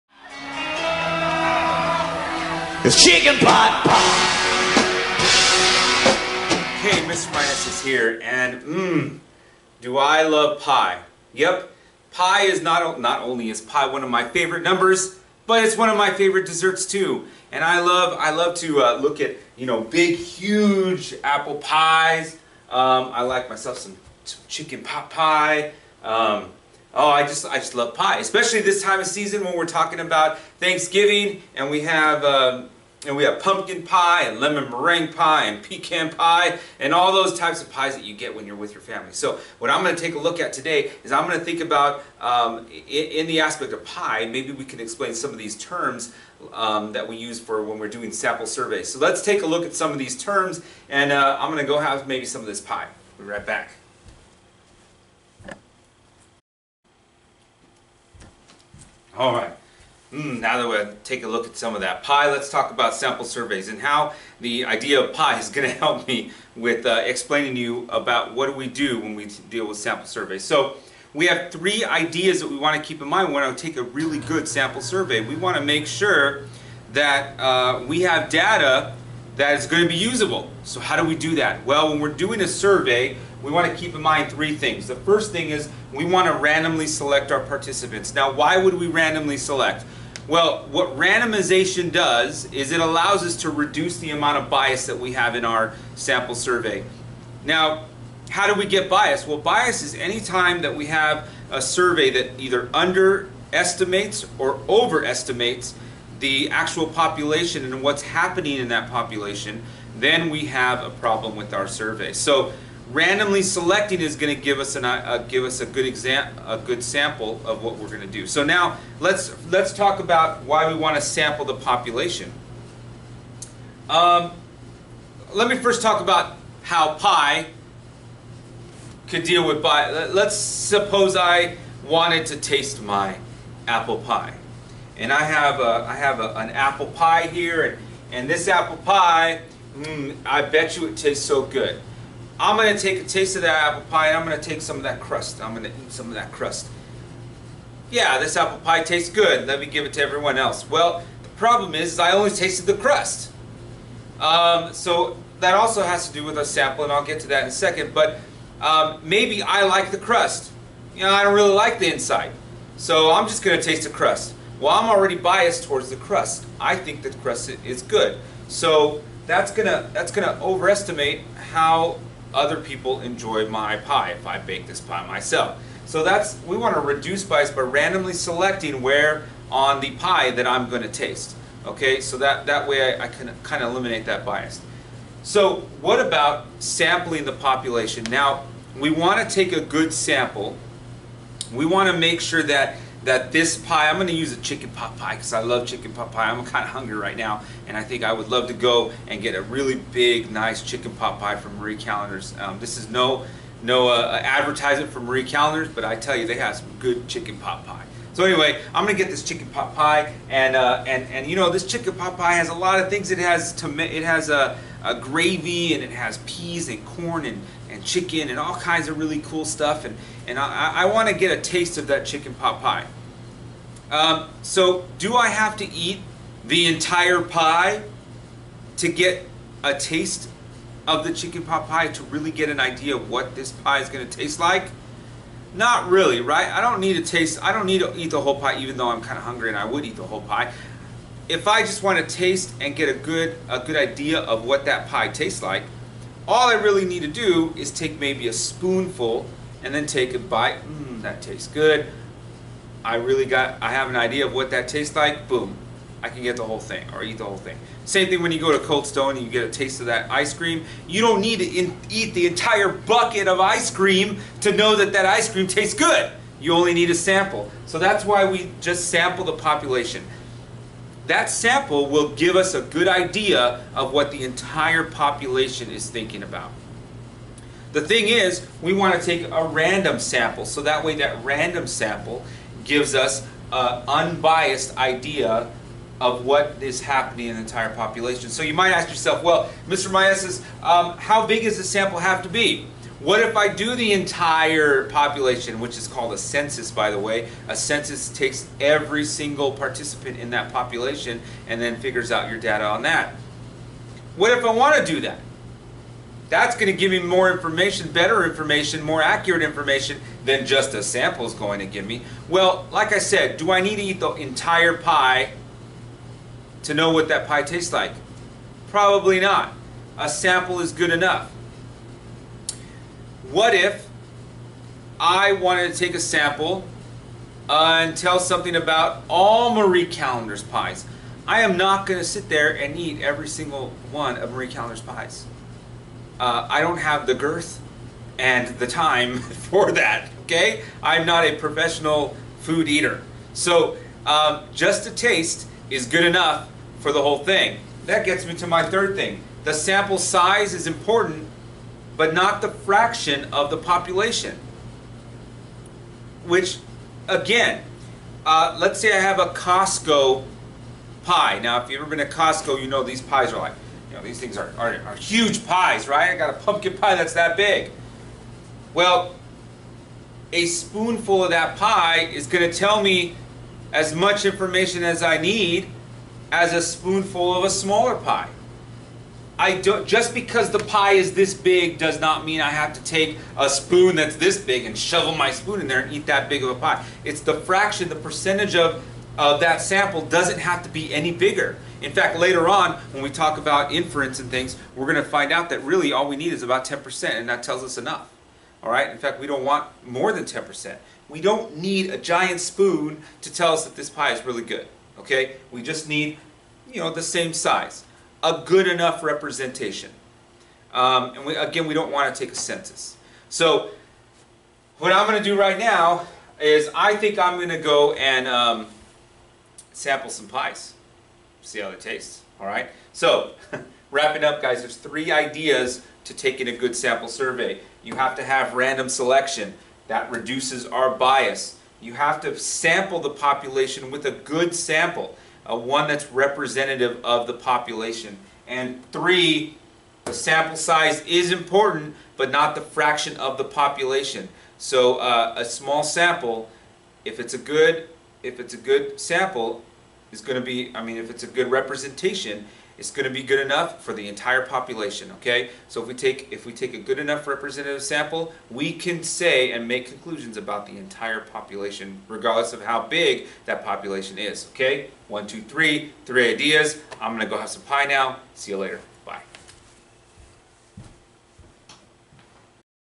AP Statistics - 3 Ideas of Sample Surveys
In this video, I discuss three important ideas to taking a Sample Survey.